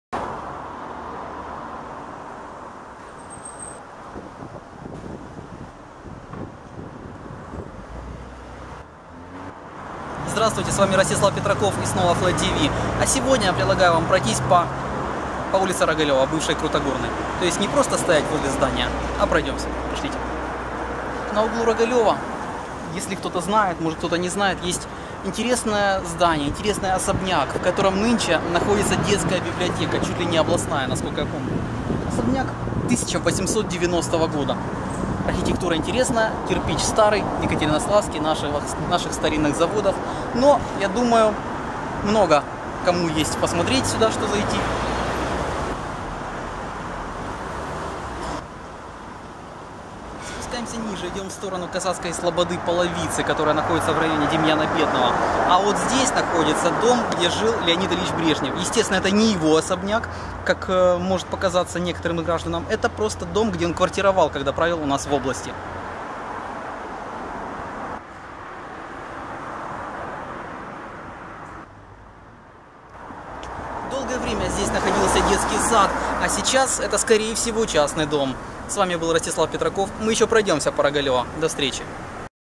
Ростислав Петраков: прогулка по улице Рогалева
Интересные и исторические места улицы Рогалева в Днепропетровске